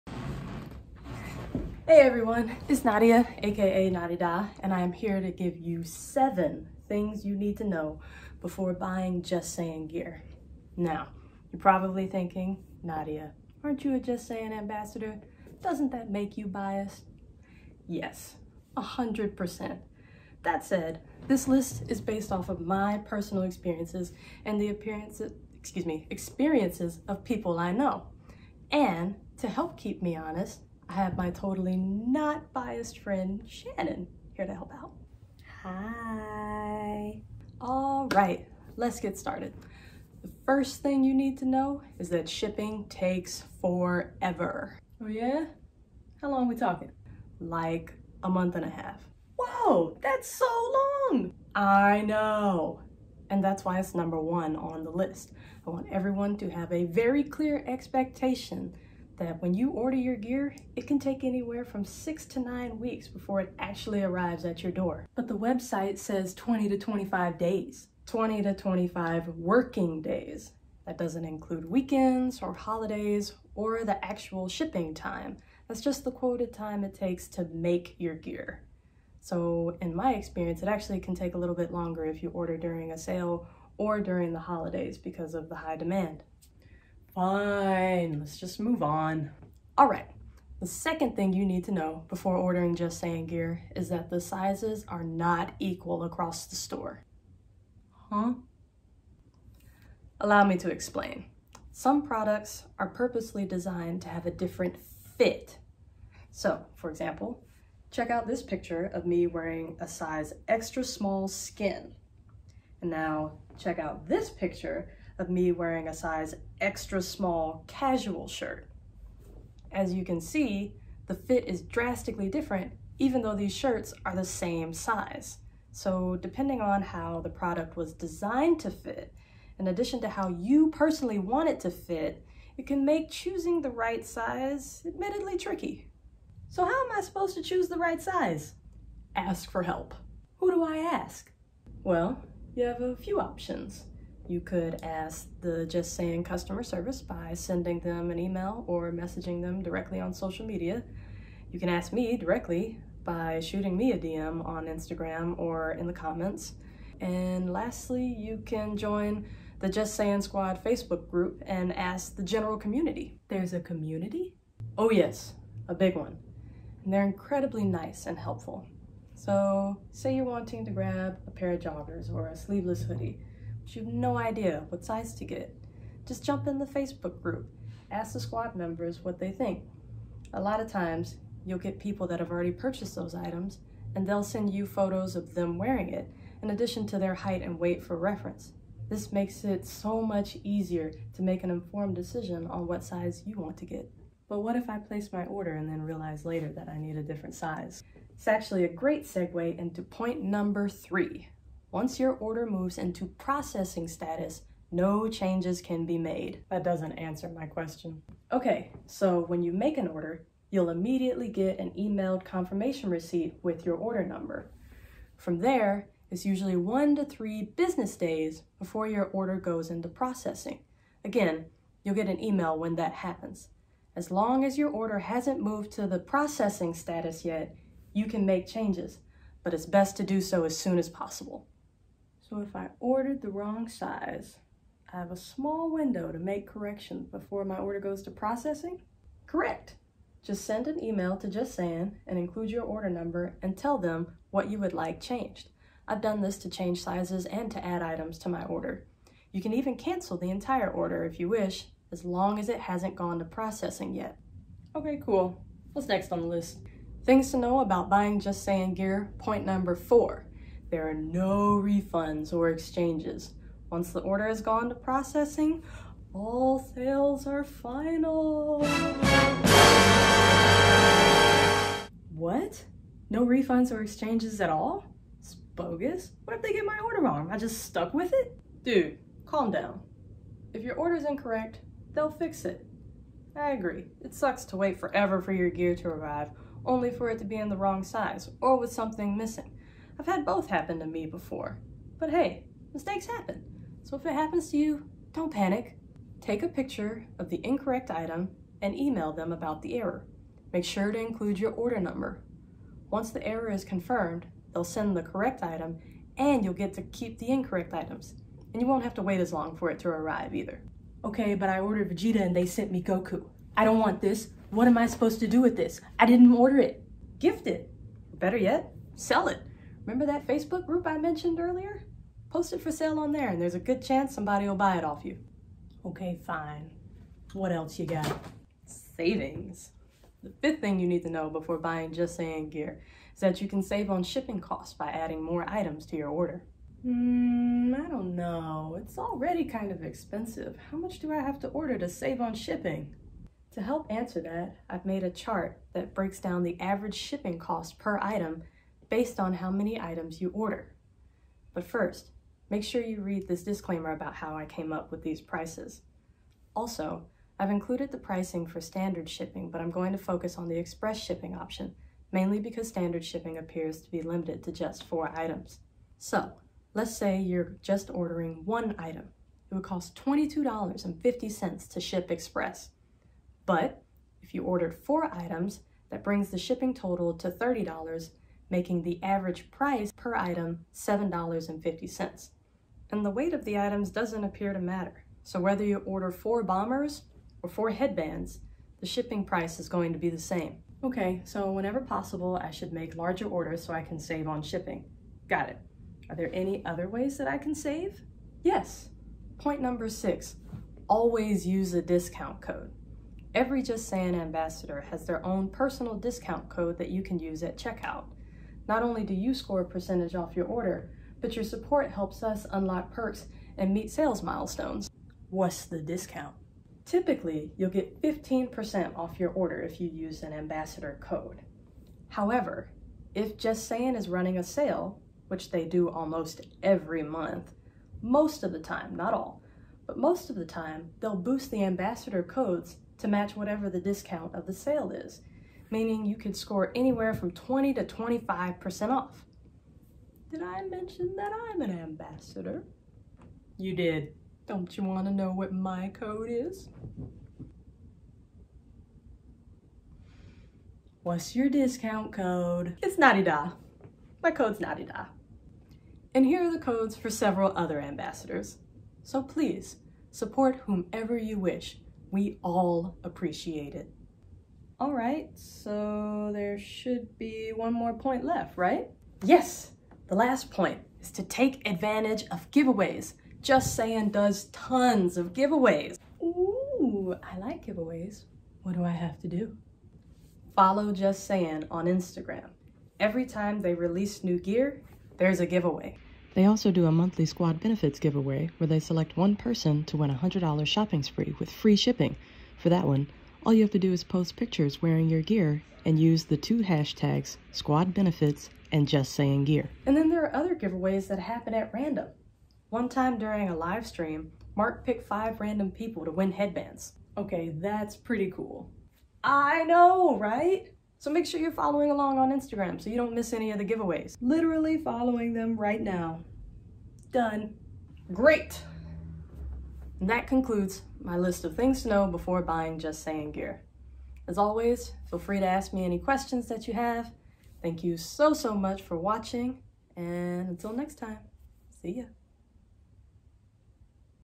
7 Things to Know Before Buying JustSaiyan Gear
This video provides insights on some things to expect for anyone who has never ordered from JustSaiyan before. Some things I talk about include:
- How to make changes to your order
- How long it takes to get delivered
- How to save money on orders
- Choosing the right size
- What to do about incorrect orders
- Winning free gear
- Joining the JustSaiyan Squad community

Purchase authentic gear only from JustSaiyan.co

Win free JustSaiyan Gear (@justsaiyan.gear) by following them on instagram and entering giveaways